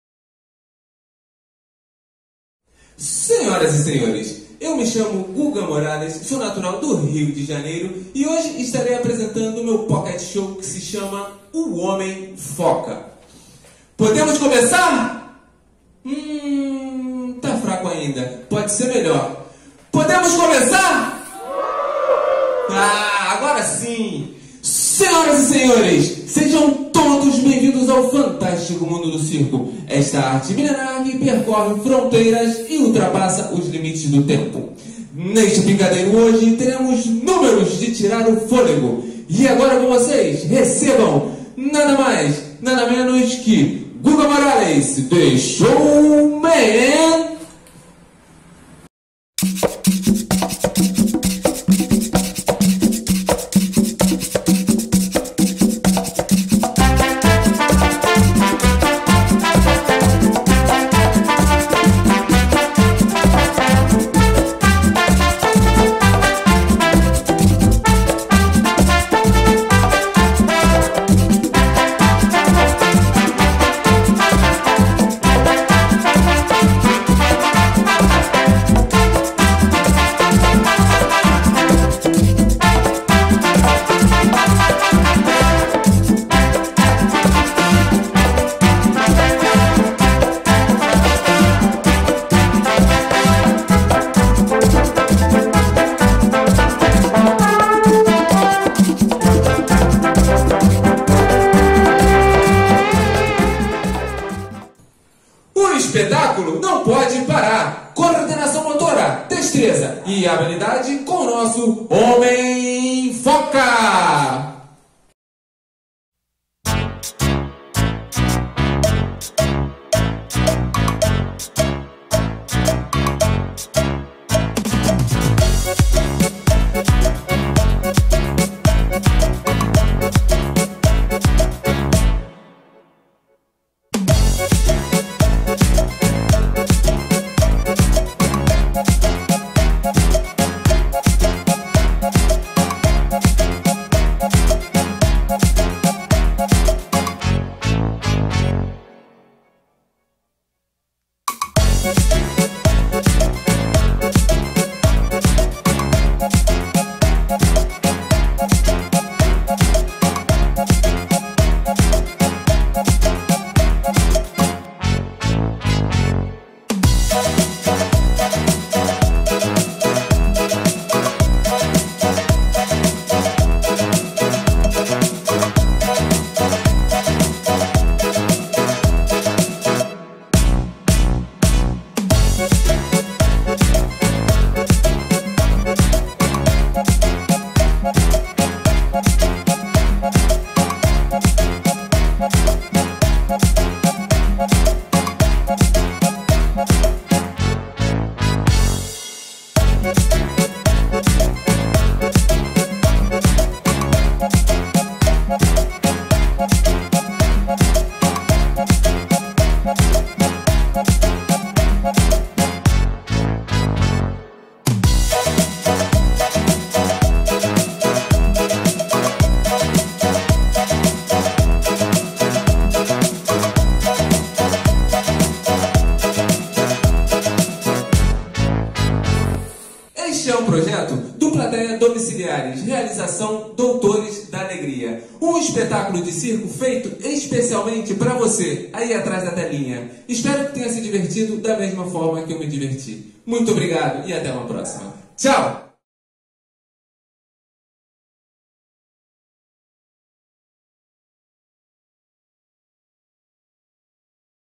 O homem foca
Para você que que está em casa, hoje tem uma apresentação de circo com malabarismos, equilibrismo e música. Pega uma pipoca e curta Guga Morales no Plateias Hospitalares em: O Homem Foca! 🤹‍♂🎪